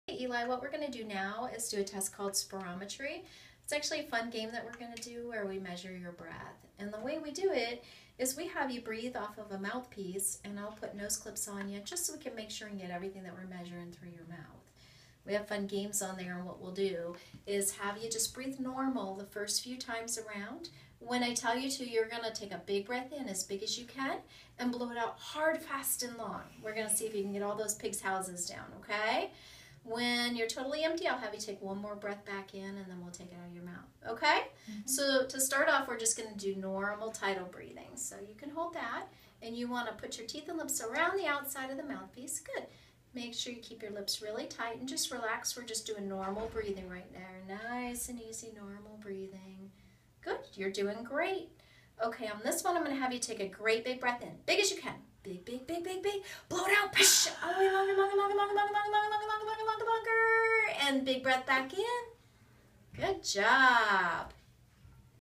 Asthma Spirometry Visit
Pediatric Associates, Inc. Asthma Educator Susan Mills guides a patient through a successful spirometry test.